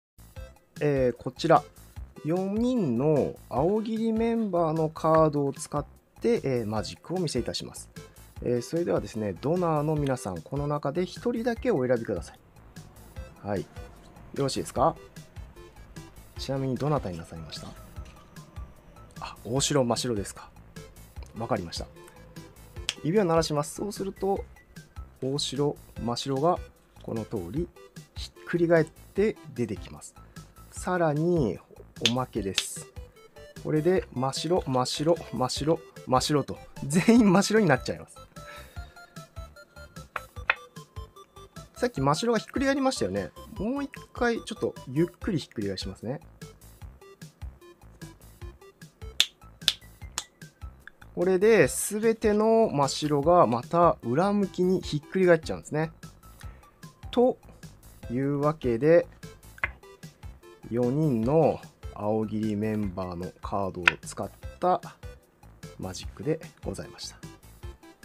大代真白推しのマジシャンによるカードマジック
大代真白はいいぞ。

一人のファンとして、早口でキモオタムーブかまそうと思ったのですが、大代のいいところを一通り書いたあと読み返したら流石にきもすぎたので消しました。この動画は最初Twitterにアップしまして、ご本人に反応をいただいたり、ドナー（大代推しの愛称）にフォローしていただいたりと、交流が産まれました。みんな、ありがとう。ありがとう。あおぎり高校はみんな好きなので、箱推ししています。